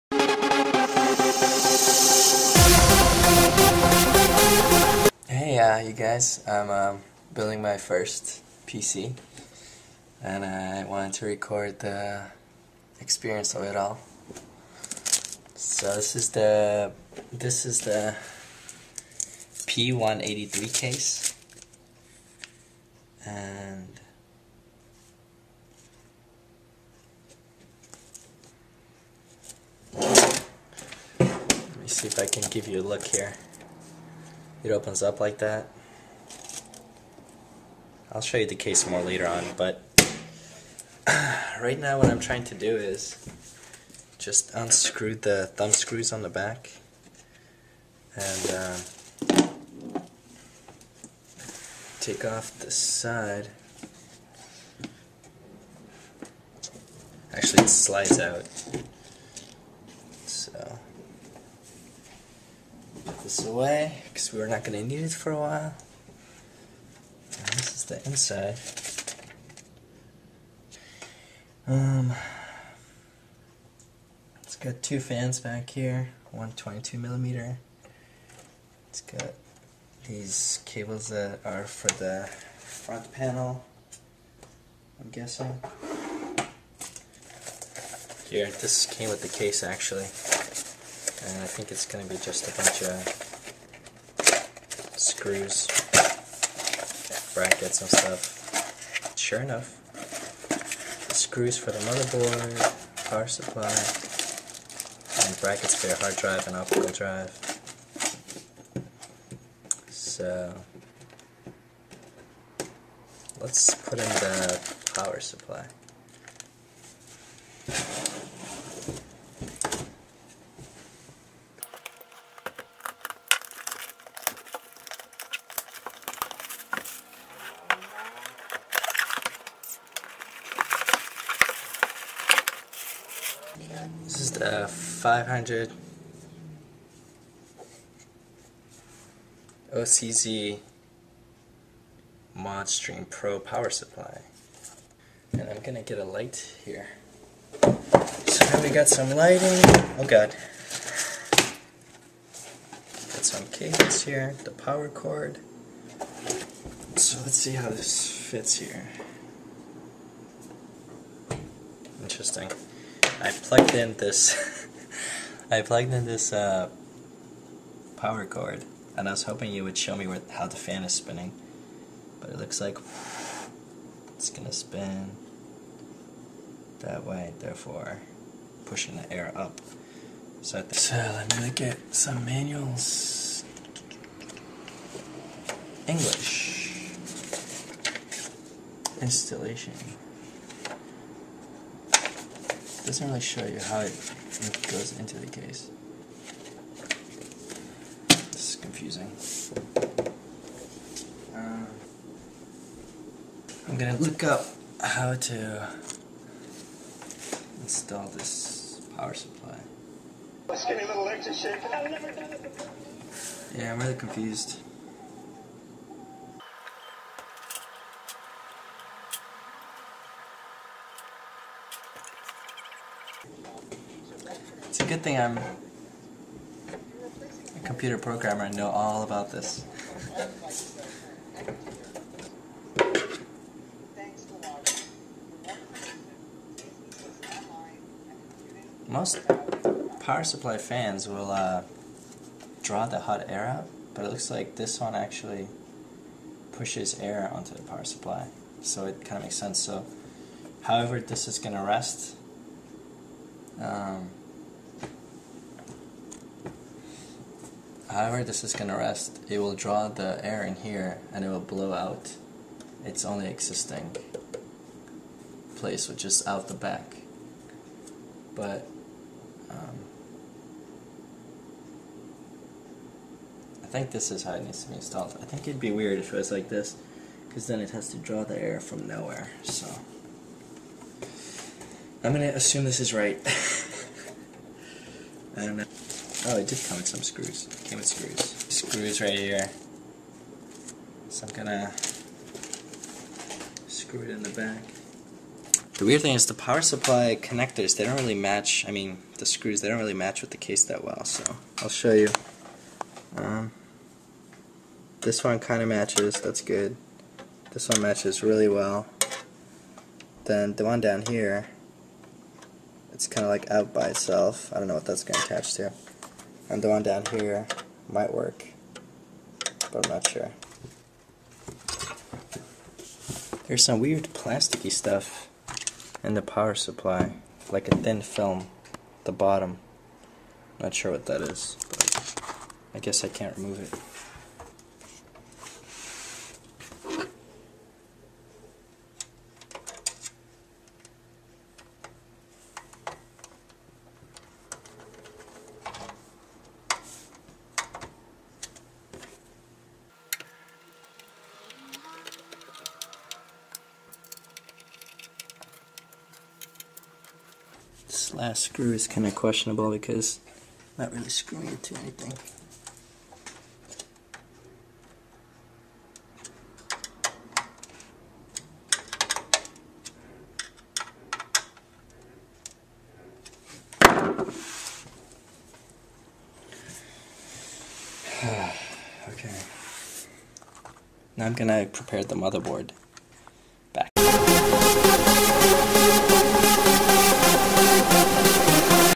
Building PC Part 1 - PSU
Part 1 was 21 minutes. Down to 6 1/2 mins. It has me figuring out how to install the power supply. I was confused because this power supply has the fan on the side of the psu instead of the back. I wanted firm documentation on how to position to install it, but instead just ended up using logic